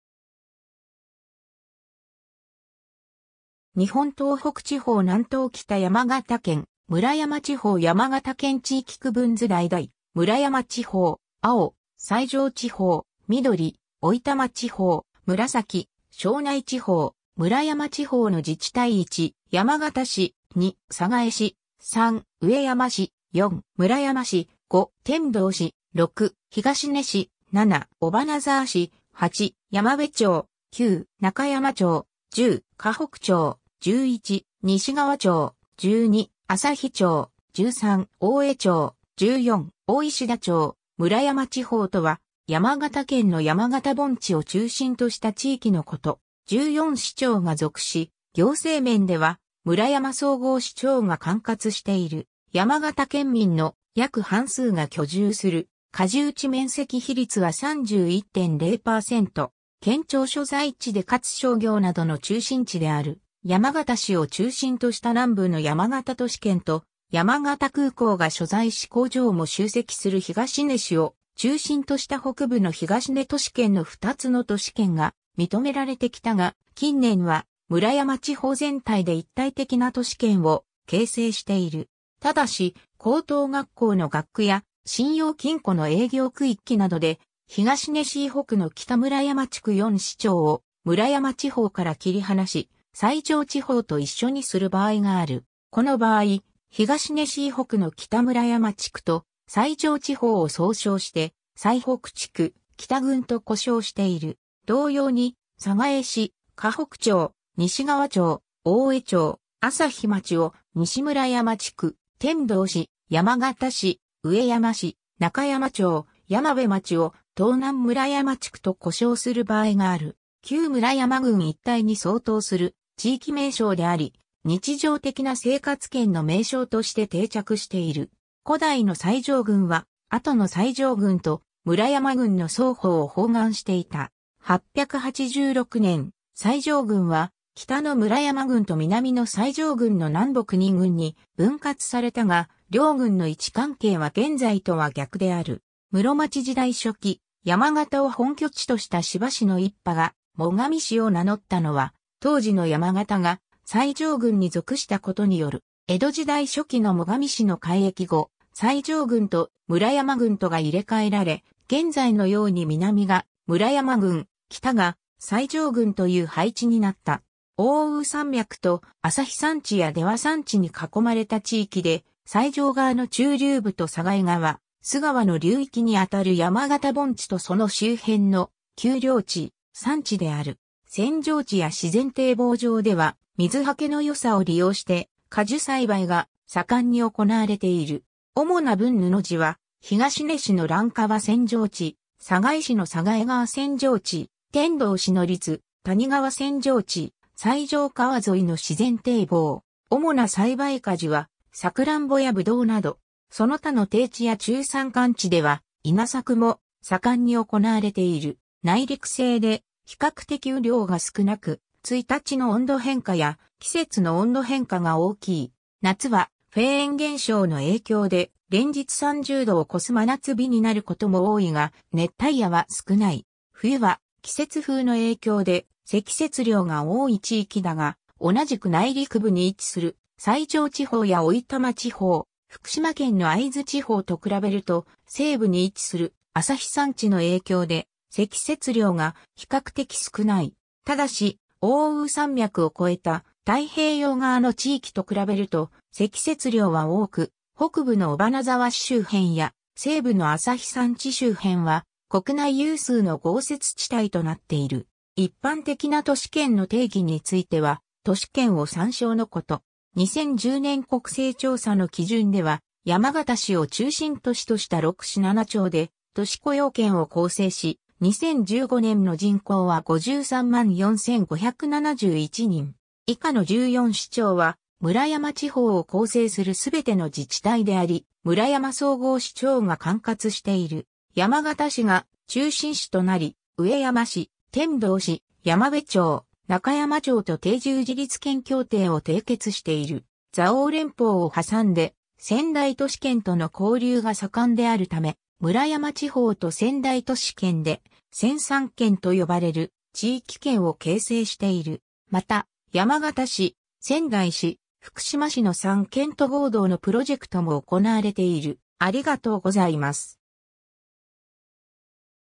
村山地方
日本 」 東北地方 」 南東北 」 山形県 」 村山地方 山形県 地域区分図 ■橙：村山地方 / ■青：最上地方 / ■緑：置賜地方 / ■紫：庄内地方 村山地方の自治体 1. 山形市 / 2. 寒河江市 / 3. 上山市 / 4. 村山市 / 5. 天童市 / 6. 東根市 / 7. 尾花沢市 / 8. 山辺町 / 9. 中山町 / 10. 河北町 / 11. 西川町 / 12. 朝日町 / 13. 大江町 / 14. 大石田町 村山地方（むらやまちほう）とは、山形県の山形盆地を中心とした地域のこと。
14市町が属し、行政面では村山総合支庁が管轄している。
山形県民の約半数が居住する。
可住地面積比率は31.0%（2009年10月1日現在）。
 県庁所在地で且つ商業などの中心地である山形市を中心とした南部の山形都市圏と、山形空港が所在し工場も集積する東根市を中心とした北部の東根都市圏の2つの都市圏が認められてきたが、近年は村山地方全体で一体的な都市圏を形成している。
 ただし、高等学校の学区や信用金庫の営業区域などで、東根市以北の北村山地区4市町を村山地方から切り離し、最上地方と一緒にする場合がある。
この場合、東根市以北の北村山地区と最上地方を総称して、最北地区、北郡（きたぐん）と呼称している。
 同様に、寒河江市、河北町、西川町、大江町、朝日町を西村山地区、天童市、山形市、上山市、中山町、山辺町を東南村山地区と呼称する場合がある。
 旧村山郡一帯に相当する地域名称であり、日常的な生活圏の名称として定着している。
古代の最上郡は、後の最上郡と村山郡の双方を包含していた。
886年（仁和2年）、最上郡は北の村山郡と南の最上郡の南北2郡に分割されたが、両郡の位置関係は現在とは逆である。
室町時代初期、山形を本拠地とした斯波氏の一派が「最上氏」を名乗ったのは、当時の山形が最上郡に属したことによる。
江戸時代初期の最上氏の改易後、最上郡と村山郡とが入れ替えられ、現在のように南が村山郡、北が最上郡という配置になった。
 奥羽山脈と、朝日山地や出羽山地に囲まれた地域で、最上川の中流部と寒河江川、須川の流域にあたる山形盆地とその周辺の丘陵地、山地である。
扇状地や自然堤防上では、水はけの良さを利用して果樹栽培が盛んに行われている。
主な分布地は、東根市の乱川扇状地、寒河江市の寒河江川扇状地、天童市の立谷川扇状地、最上川沿いの自然堤防。
主な栽培果樹はサクランボやブドウなど。
その他の低地や中山間地では、稲作も盛んに行われている。
 内陸性で比較的雨量が少なく、1日の温度変化や、季節の温度変化が大きい。
夏はフェーン現象の影響で連日30度を越す真夏日になる事も多いが、熱帯夜は少ない。
冬は季節風の影響で積雪量が多い地域だが、同じく内陸部に位置する最上地方や置賜地方、福島県の会津地方と比べると、西部に位置する朝日山地の影響で、積雪量が比較的少ない。
但し、奥羽山脈を越えた太平洋側の地域と比べると積雪量は多く、北部の尾花沢市周辺や西部の朝日山地周辺（西川町・大江町・朝日町の山間部など）は国内有数の豪雪地帯となっている。
 一般的な都市圏の定義については都市圏を参照のこと。
 2010年国勢調査の基準では山形市を中心都市とした6市7町で都市雇用圏を構成し、2015年の人口は534,571人。
以下の14市町は、村山地方を構成する全ての自治体であり、村山総合支庁が管轄している。
 山形市が中心市となり、上山市、天童市、山辺町、中山町と定住自立圏協定を締結している。
 蔵王連峰を挟んで仙台都市圏との交流が盛んであるため、村山地方と仙台都市圏で「仙山圏」と呼ばれる地域圏を形成している。
また、山形市・仙台市・福島市の三県都合同のプロジェクトも行われている（→仙台経済圏）。